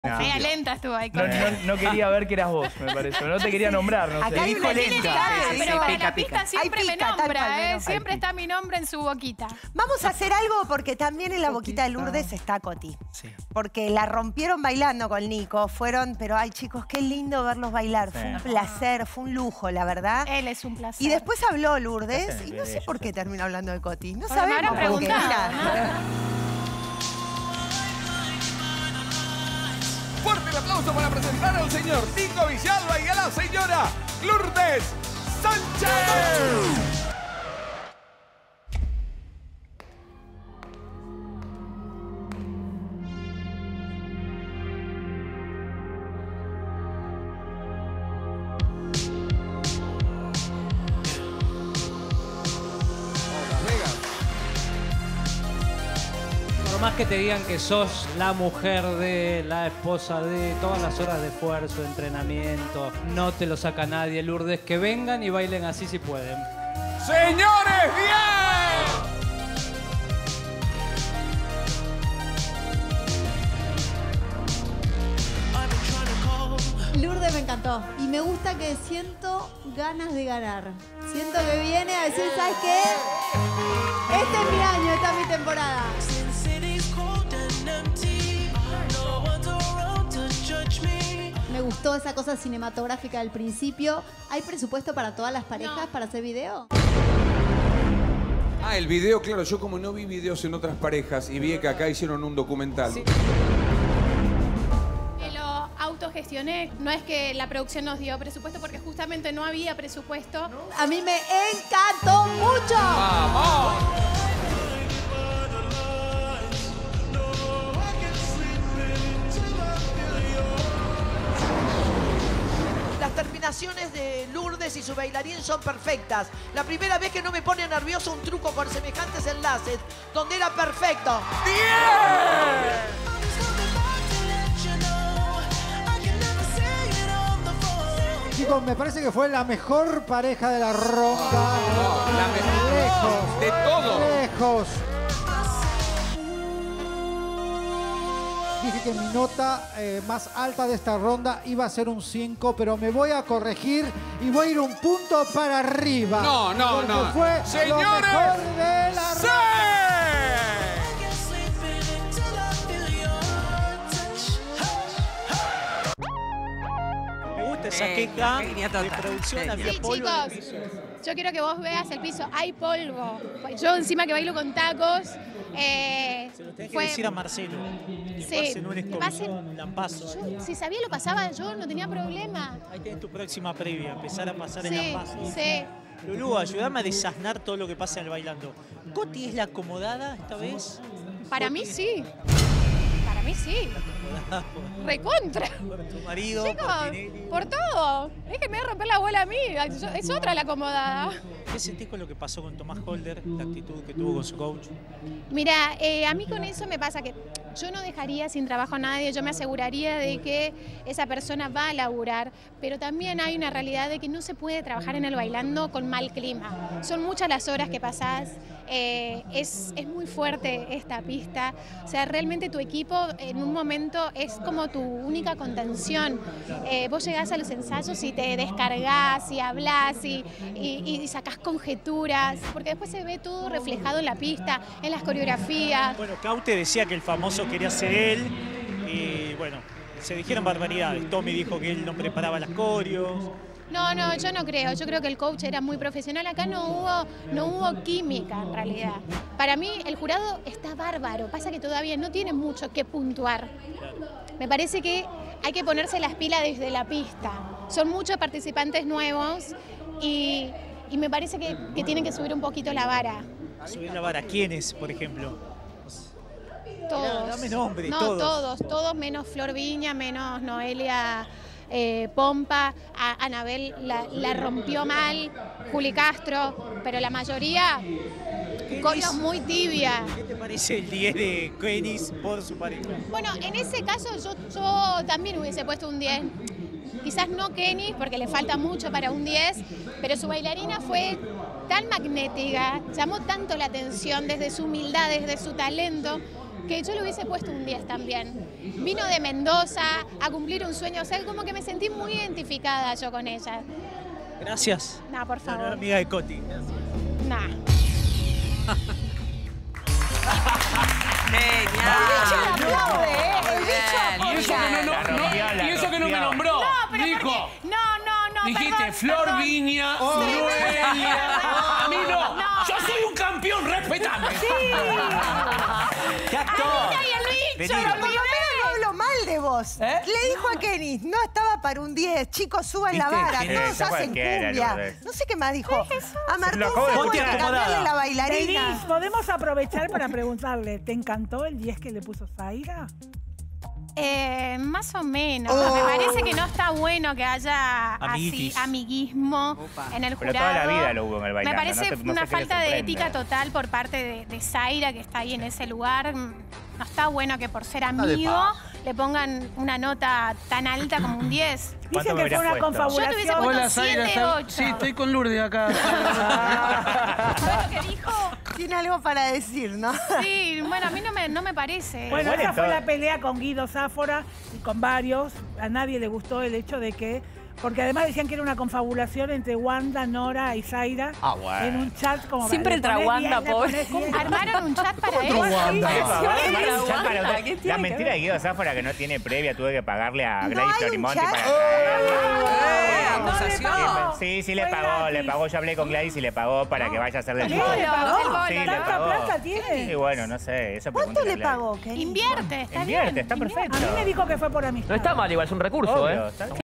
📢 Lourdes Sánchez: "Coty es la acomodada"
💬 "A Kennys le falta mucho para el 10"

💬 "Lo de Zaira fue una falta de ética"